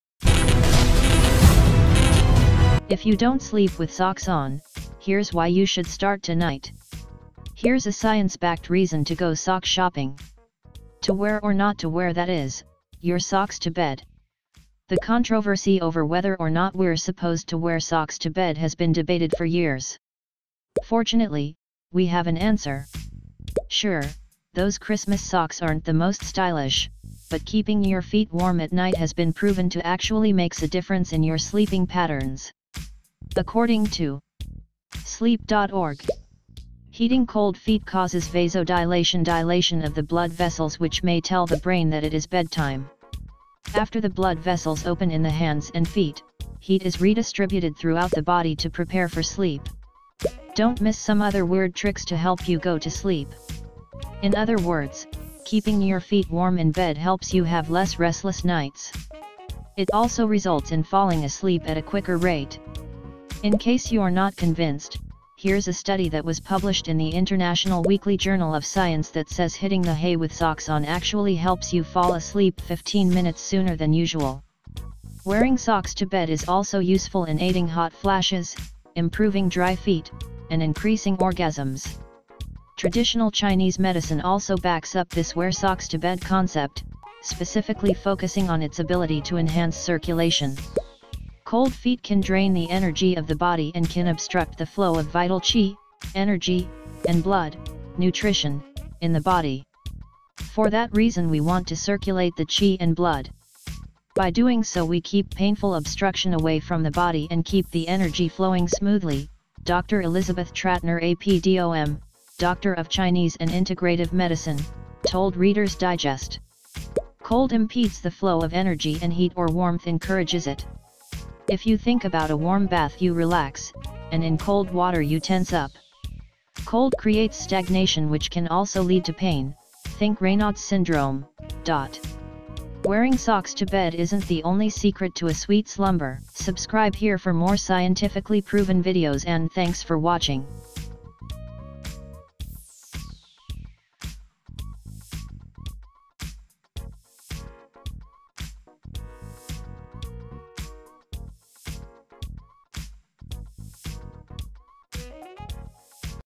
If You Don’t Sleep with Socks On, Here’s Why You Should Start Tonight || Scientifically Proven
If You Don’t Sleep with Socks On, Here’s Why You Should Start Tonight
Here's a science-backed reason to go sock shopping!
To wear or not to wear—that is, your socks to bed. The controversy over whether or not we’re supposed to wear socks to bed has been debated for years. Fortunately, we have an answer.

Sure, those Christmas socks aren’t the most stylish, but keeping your feet warm at night has been proven to actually makes a difference in your sleeping patterns. According to
, “Heating cold feet causes vasodilation—dilation of the blood vessels—which may tell the brain that it is bedtime. After the blood vessels open in the hands and feet, heat is redistributed throughout the body to prepare for sleep.” (Don’t miss some other weird tricks to help you go to sleep.)

In other words, keeping your feet warm in bed helps you have less restless nights. It also results in falling asleep at a quicker rate. In case you’re not convinced, here’s a study that was published in the International Weekly Journal of Science that says hitting the hay with socks on actually helps you fall asleep fifteen minutes sooner than usual. Wearing socks to bed is also useful in aiding hot flashes, improving dry feet, and increasing orgasms.

Traditional Chinese medicine also backs up this wear-socks-to-bed concept, specifically focusing on its ability to enhance circulation. “Cold feet can drain the energy of the body and can obstruct the flow of vital qi (energy) and blood (nutrition) in the body. For that reason we want to circulate the ‘qi and blood.’ By doing so we keep painful obstruction away from the body and keep the energy flowing smoothly,” Dr. Elizabeth Trattner A.P. DOM, Doctor of Chinese and Integrative Medicine, told Reader’s Digest. “Cold impedes the flow of energy and heat or warmth encourages it. If you think about a warm bath you relax, and in cold water you tense up. Cold creates stagnation which can also lead to pain (think Reynaud’s Syndrome).”

Wearing socks to bed isn’t the only secret to a sweet slumber.